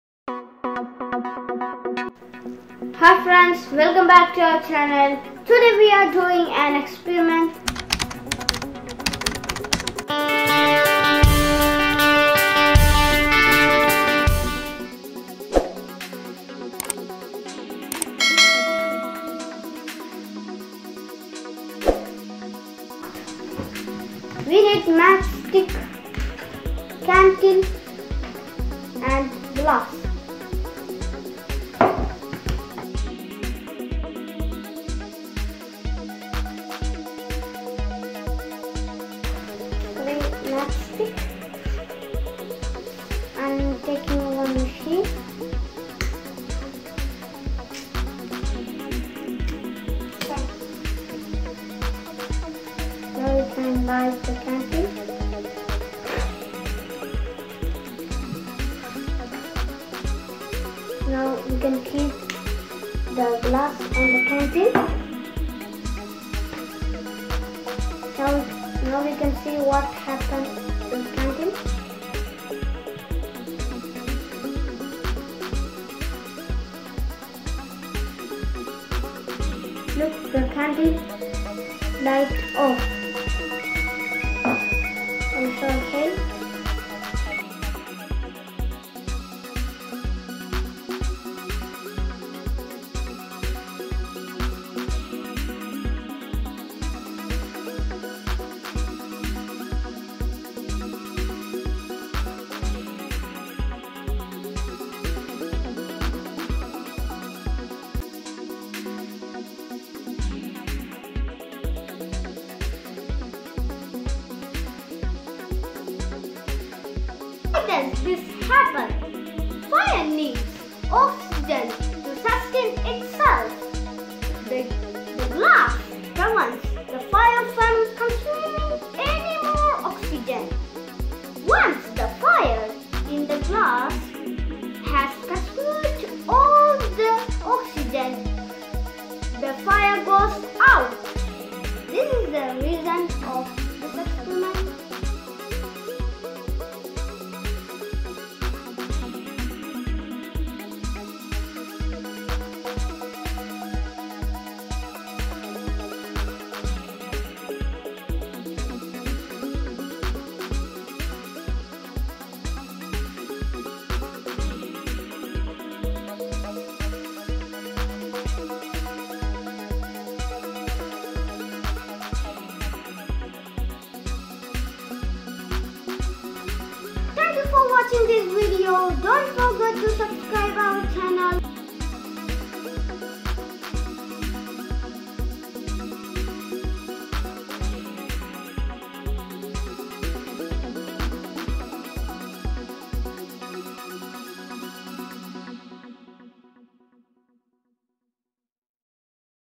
Experiment with Candle - with Reason
This is an Experiment Video..
Fire Needs Oxygen to Sustain ...